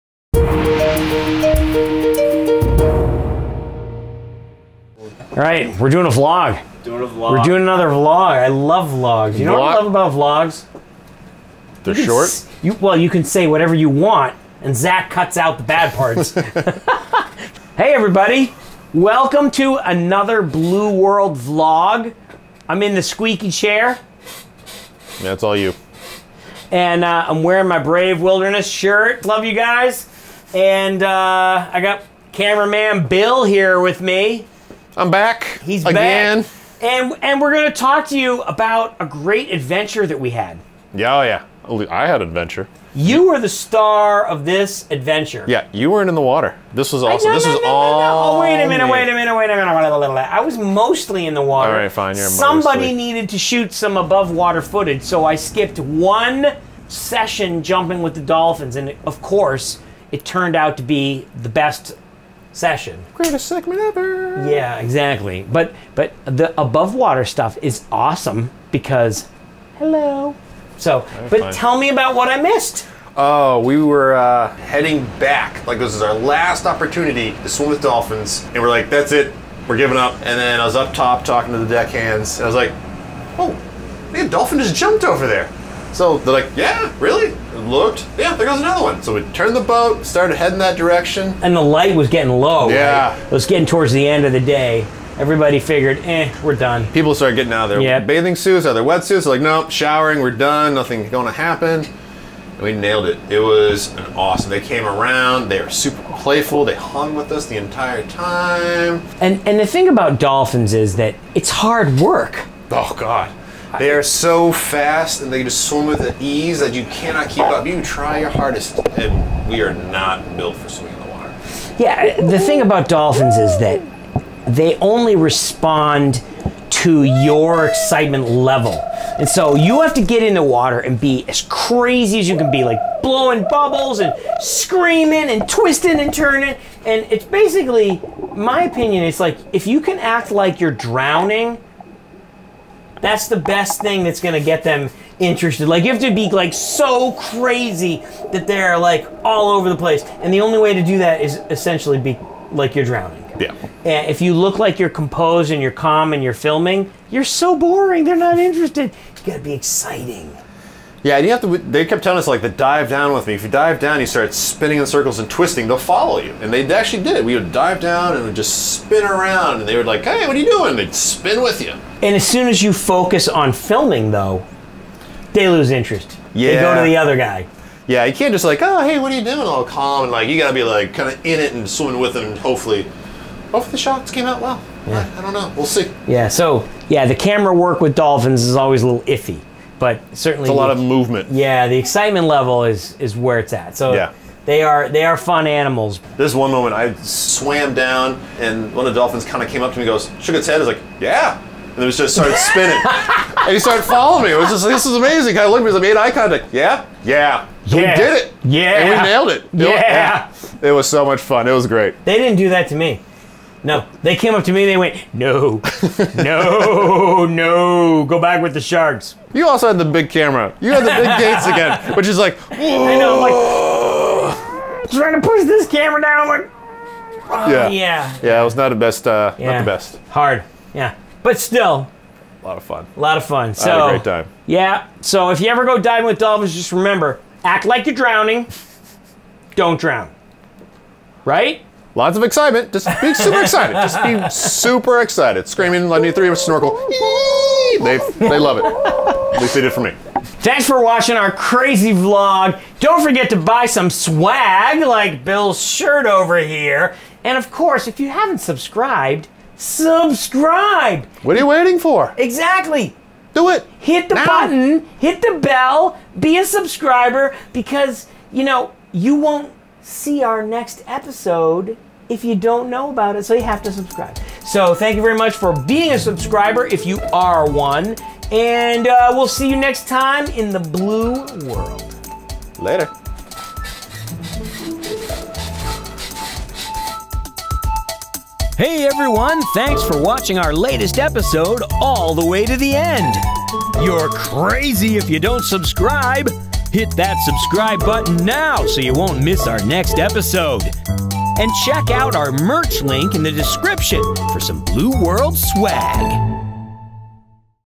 Swimming with Dolphins | BLUE WORLD VLOG
Jonathan Bird and "Cameraman Bill" talk about what it's like to swim with wild Spotted Dolphins.  You can watch the full Blue World episode about the dolphins on our main channel here: